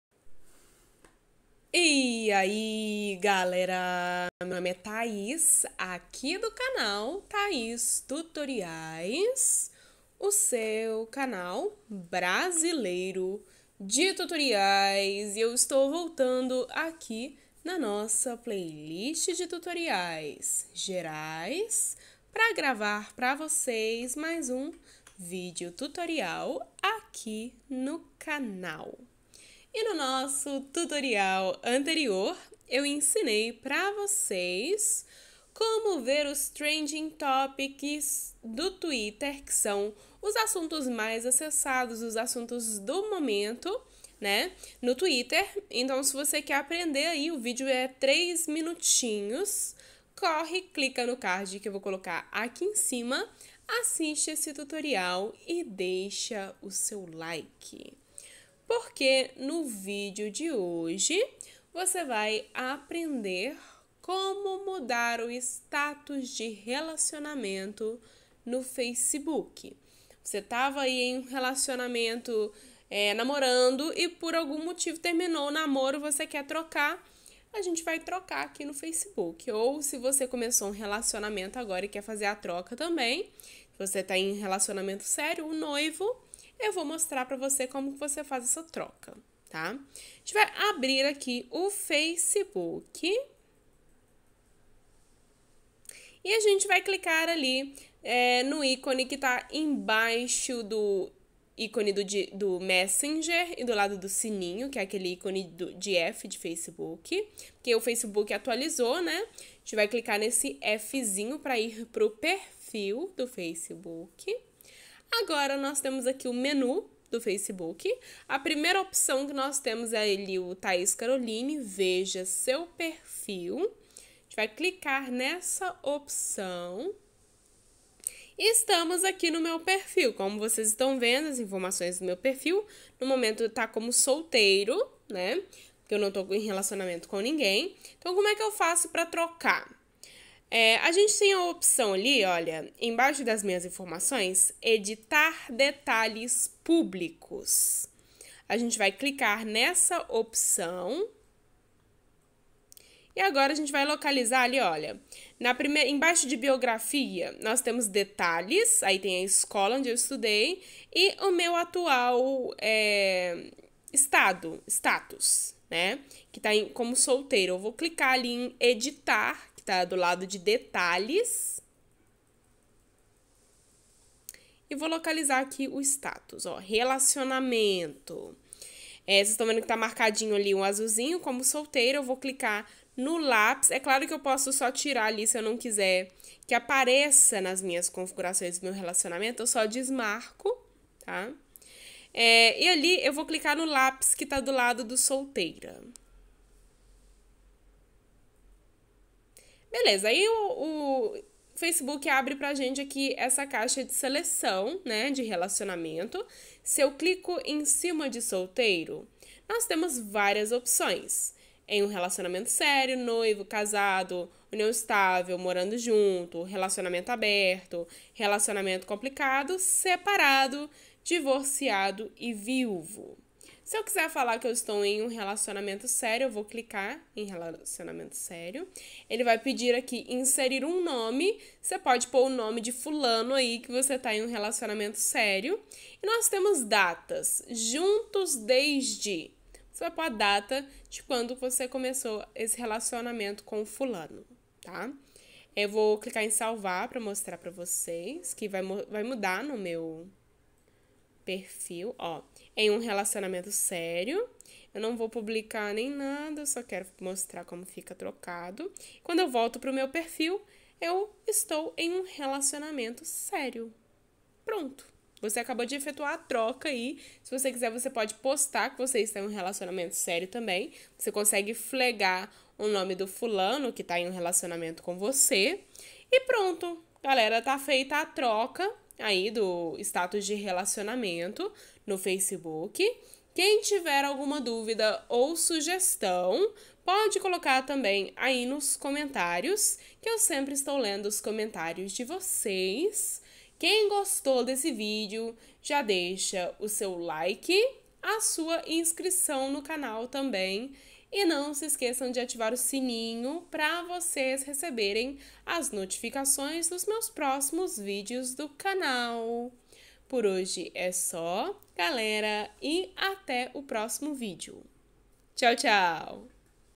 COMO MUDAR O STATUS DE RELACIONAMENTO NO FACEBOOK
COMO MUDAR O STATUS DE RELACIONAMENTO NO FACEBOOK, este vídeo é um tutorial para você aprender facilmente como mudar o status de relacionamento no facebook,